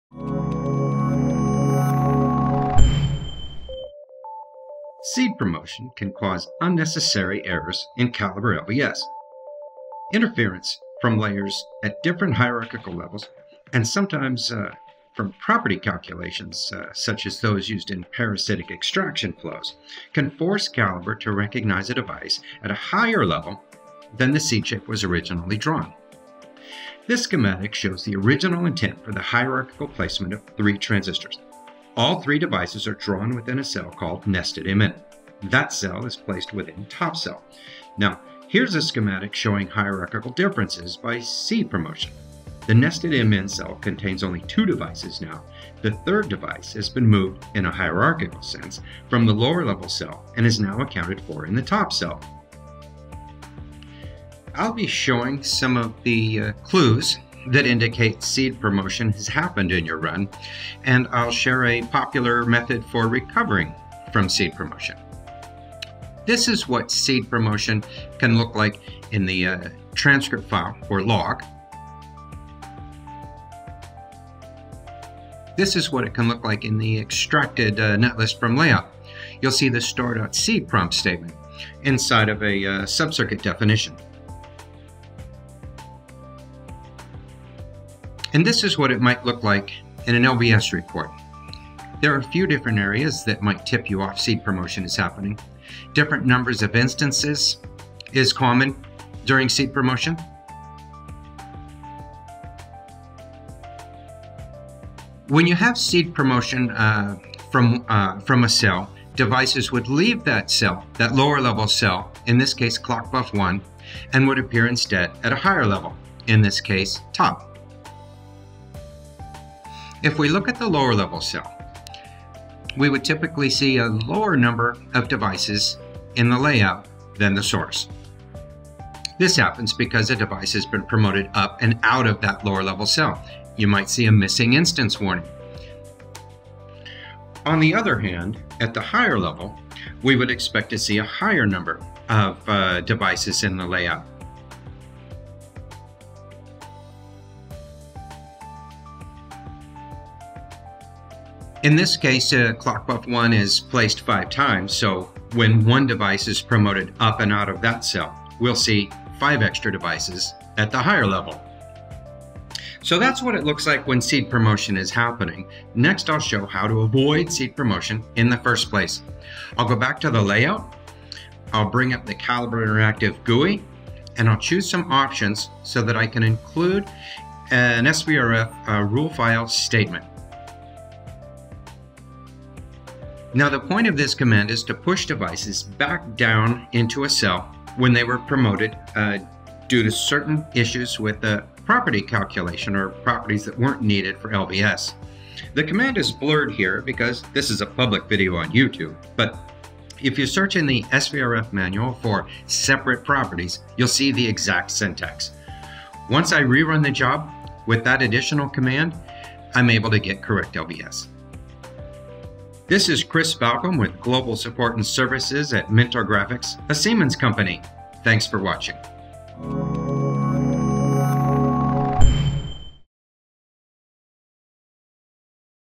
How to deal with Calibre LVS "Seed Promotion"
Seed promotion” happens when Calibre LVS is forced to form devices at a higher level than expected and can lead to unnecessary LVS errors. There are many possible causes and multiple solutions that can be explored. This video shows one way to recover from seed promotion which is easy to use and effective in a majority of use-cases.